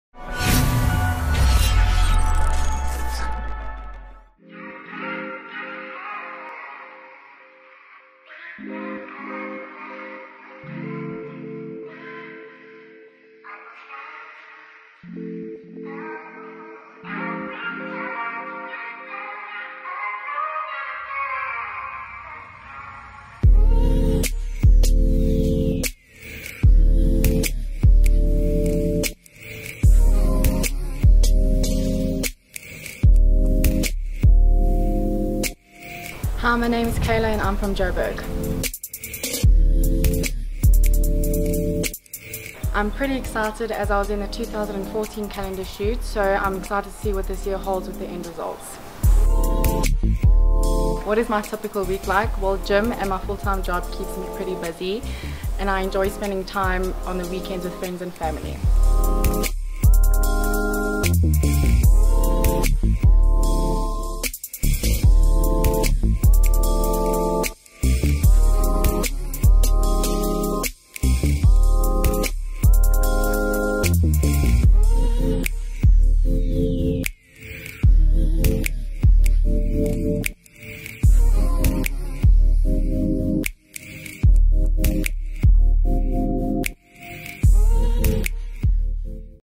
Miss December 2017 Calendar Girl Kayla Grover
Go behind the scenes on our 2017 LW Mag Calendar shoot with our Miss December Calendar Girl, Kayla Grover.

Key Make-up Artist and Hair Stylist: Morag Steyn assisted by Sonam Chettiar and Tilana Den Hartog – Catrice Cosmetics | Caribbean Tan.
Calendar Girls dressed by Jeannie Bikini.
Filmed and edited by Obsession Media House.
Photography by Hotlens Photographic.
Location: Pretville.